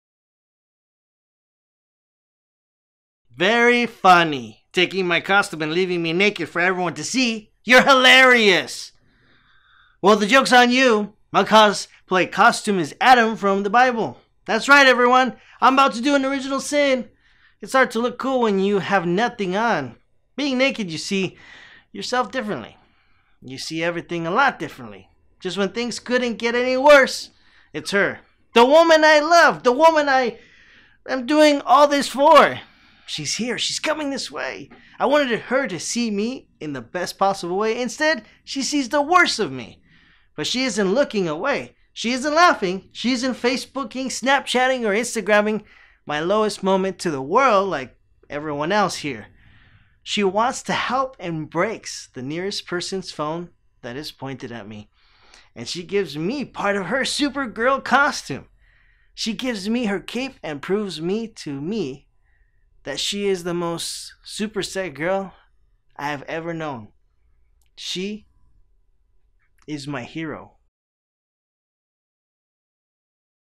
Goodbye to Neverland Part 5 free monologue for male
presents a series of monologues for male actors about a guy who doesn't want to grow up. "Goodbye to Neverland" is written by D. M. Larson

(RANDY hides behind his box - lights can go down - When lights come up , he comes up with nothing on and is behind the box/suitcase/trunk. Randy appears to be naked to the audience)

RANDY
Very funny.  Taking my costume and leaving me naked for everyone to see. - you’re hilarious!  Well, the jokes on you.  My cosplay costume is Adam from the Bible.  That’s right everyone. I’m about to do an original sin…!

It’s hard to look cool when you have nothing on.  Being naked… you see yourself differently… you see everything a lot differently… Just when things couldn't get any worse… it’s her, the woman I loved, the woman I doing all this for… she’s here… she’s coming this way!  I wanted her to see me in the best possible way. Instead she sees the worst of me. But she isn't looking away… she isn’t laughing… she isn't facebooking, snapchatting or instagramming my lowest moment to the world like everyone else here.  She wants to help and breaks the nearest person’s phone that is pointed at me… and she gives me part of her super girl costume. She gives me her cape and proves me to me that she is the most superset girl I have ever known. She is my hero.

END OF MONOLOGUE 5

Performed by Joey Perez West